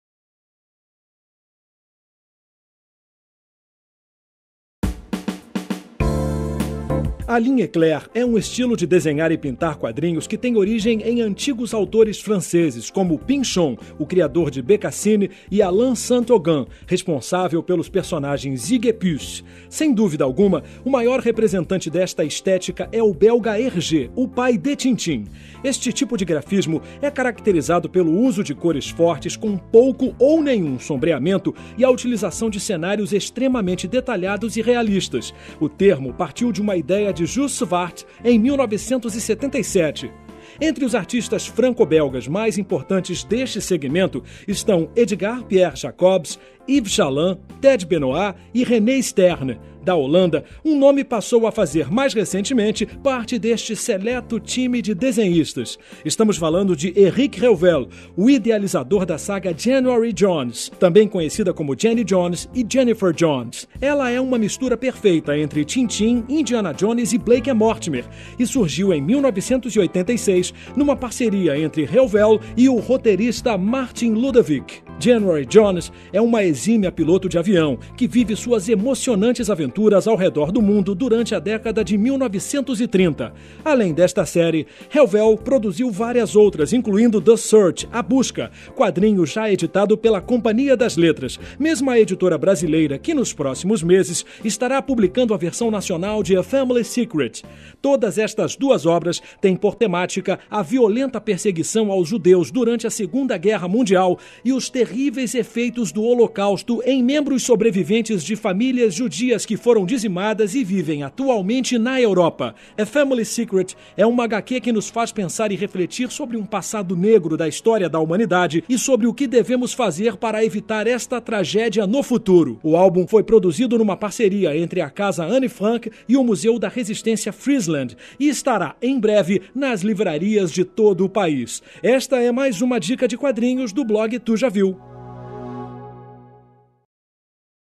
Os quadrinhos Ligne Claire de Eric Heuvel.
Matéria do blog Tujaviu sobre a Ligne Claire, um jeito de desenhar e pintar quadrinhos que lembra o estilo de Tintin.